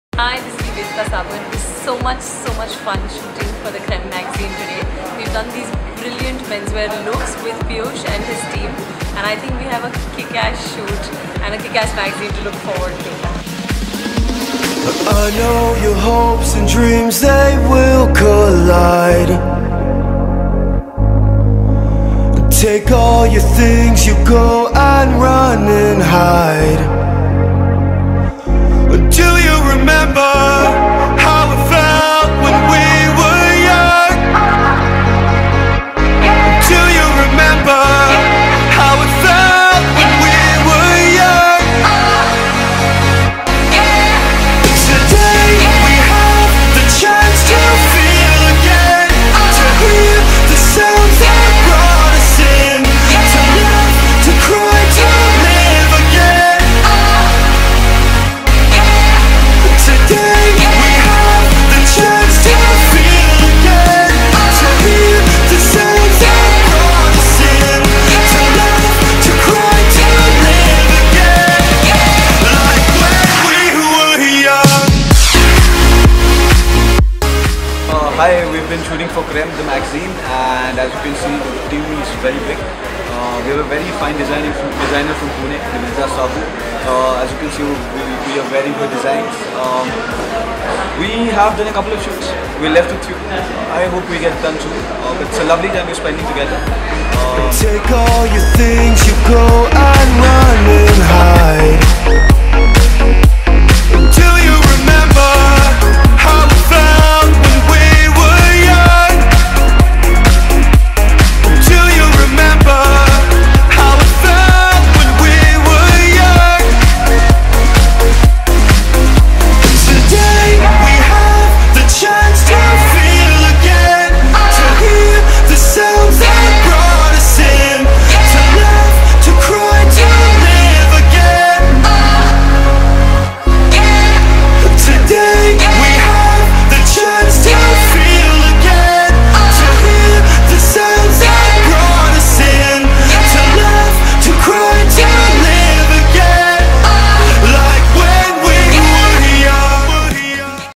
Piyush upadhyay/Nivedita Saboo Couture
Planned Edited and Shot Behind the scene with Nivedita Saboo for her Fashion shoot